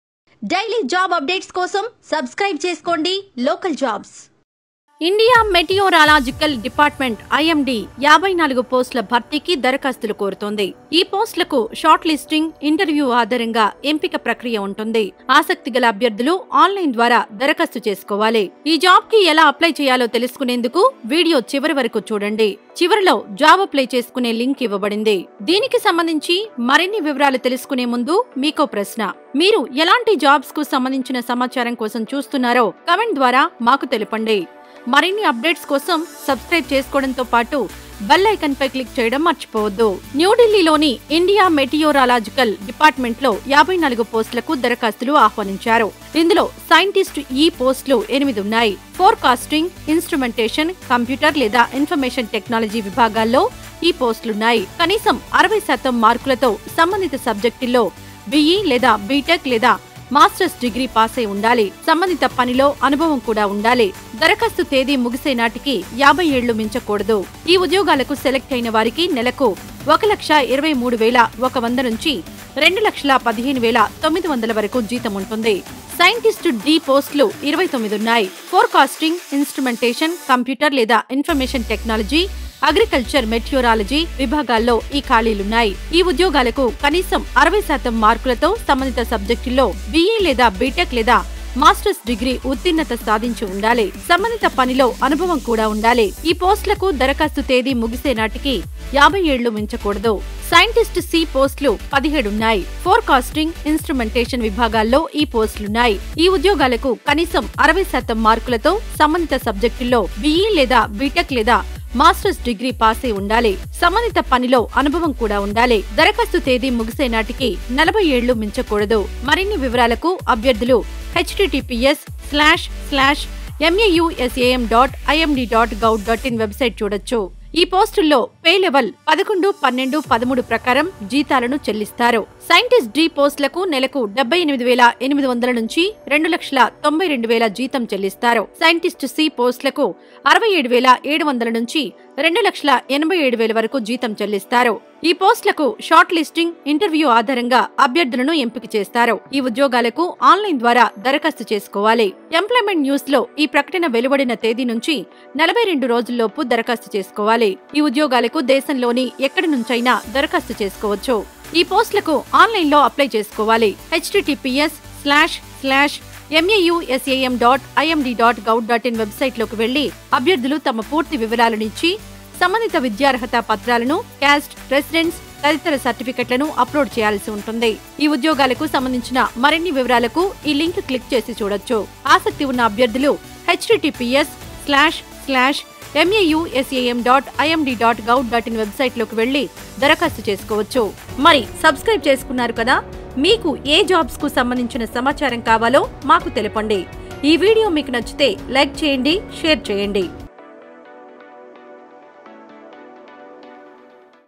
లక్షల్లో జీతం, ఇంటర్వ్యూ ఆధారంగా ఎంపిక | ఐఎండీలో ఉద్యోగ అవకాశాలు | Job opportunities at IMD
ఐఎండీలో ఉద్యోగ అవకాశాలు
లక్షల్లో జీతం, ఇంటర్వ్యూ ఆధారంగా ఎంపిక

న్యూఢిల్లీలోని ఇండియా మెటియోరాలాజికల్ డిపార్ట్‌మెంట్‌లో 54 పోస్టులకు దరఖాస్తులు ఆహ్వానించడం జరుగుతుంది. ఇందులో సైంటిస్ట్‌-ఈ పోస్టులు 8 ఉన్నాయి. ఫోర్‌కాస్టింగ్‌, ఇనుస్ట్రుమెంటేష‌న్‌, కంప్యూట‌ర్‌ లేదా ఇన్ఫ‌ర్మేష‌న్ టెక్నాల‌జీ విభాగాల్లో ఈ పోస్టులు ఉన్నాయి. క‌నీసం 60 శాతం మార్కుల‌తో సంబంధిత సబ్జెక్టుల్లో బీఈ లేదా బీటెక్‌ లేదా మాస్ట‌ర్స్ డిగ్రీ పాసై ఉండాలి. సంబంధిత ప‌నిలో అనుభ‌వం ఉండాలి. దరఖాస్తు తేదీ ముగిసే నాటికి 50 ఏళ్లు మించకూడదు. సైంటిస్ట్‌-డీ పోస్టులు 29 ఉన్నాయి. ఫోర్‌కాస్టింగ్‌, ఇనుస్ట్రుమెంటేష‌న్‌, కంప్యూట‌ర్‌ లేదా ఇన్ఫ‌ర్మేష‌న్ టెక్నాల‌జీ, అగ్రిక‌ల్చ‌ర్ మెటీయోరాల‌జి విభాగాల్లో ఈ ఖాళీలున్నాయి. ఈ ఉద్యోగాలకు కనీసం 60 శాతం మార్కులతో సంబంధిత సబ్జెక్టుల్లో బీఈ లేదా బీటెక్ లేదా మాస్టర్స్ డిగ్రీ ఉత్తీర్ణత సాధించి ఉండాలి. సంబంధిత ప‌నిలో అనుభ‌వం కూడా ఉండాలి. ఈ పోస్టులకు ద‌ర‌ఖాస్తు తేదీ ముగిసే నాటికి 50 ఏళ్లు మించకూడదు. సైంటిస్ట్ సీ పోస్టులు 17 ఉన్నాయి. ఫోర్ కాస్టింగ్, ఇనుస్ట్రుమెంటేష‌న్ విభాగాల్లో ఈ పోస్టులున్నాయి. ఈ ఉద్యోగాలకు కనీసం 60 శాతం మార్కుల‌తో సంబంధిత స‌బ్జెక్టుల్లో బీఈ లేదా బీటెక్‌ లేదా మాస్టర్స్ డిగ్రీ పాసై ఉండాలి. సంబంధిత పనిలో అనుభవం కూడా ఉండాలి. దరఖాస్తు తేదీ ముగిసే నాటికి 40 ఏళ్లు మించ‌కూడ‌దు. సైంటిస్ట్ డీ పోస్టులకు నెలకు రూ.78800ల నుంచి రూ.209200లు జీతం చెల్లించడం జరుగుతుంది. సైటిస్ట్ సీ పోస్టులకు రూ. 67700ల నుంచి రూ.208700ల వరకూ చెల్లిస్తారు. ఈ పోస్టులకు షార్ట్ లిస్టింగ్‌, ఇంటర్వ్యూ ఆధారంగా అభ్యర్థులను ఎంపిక చేయడం జరుగుతుంది. ఈ ఉద్యోగాలకు ఆన్‌లైన్ ద్వారా ద‌ర‌ఖాస్తు చేసుకోవాలి. ఎంప్లాయిమెంట్ న్యూస్‌లో ఈ ప్ర‌క‌ట‌న వెలువ‌డిన తేదీ నుంచి 42 రోజుల్లోపు ద‌ర‌ఖాస్తు చేసుకోవాలి. ఈ ఉద్యోగాలకు సంబంధించిన మరిన్ని వివరాలకు వెబ్‌సైట్‌లోకి తెలుసుకోవచ్చు.

Job opportunities at IMD
Salary in lakhs, selection based on interview

Applications are invited for 54 posts in India Meteorological Department, New Delhi. This includes Scientist-8 posts. These posts are in the fields of Forecasting, Instrumentation, Computer or Information Technology. Must have BE or BTech or Masters degree pass in relevant subjects with at least 60% marks. Must have experience in related work. Must not exceed 50 years by the end of the application date. There are 29 Scientist-D posts. These vacancies are in the fields of Forecasting, Instrumentation, Computer or Information Technology, Agricultural Meteorology. Must have passed BE or BTech or Masters degree in relevant subjects with at least 60 per cent marks for these jobs. Must also have experience in related work. The application deadline for these posts should not exceed 50 years. There are 17 Scientist Sea posts. These posts are in the Forecasting and Instrumentation sections. Must have BE or BTech or Master's degree in relevant subjects with at least 60% marks for these jobs. Must also have experience in related work. Not to exceed 40 years from the date of application. Scientist D posts range in salary from Rs 78,800 to Rs 20,9200 per month. Rs. 67700 to Rs.208700. Candidates will be shortlisted for these posts and selected on the basis of interview. These jobs need to be applied for online. Must apply within 42 days from the date of publication of this announcement in Employment News. Further details on these jobs can be found

Related Posts -

Related Queries -
1.imd jobs 2021
2. imd
3. imd recruitment 2021
4. india metrological department
5. imd recruitment
6. meteorological department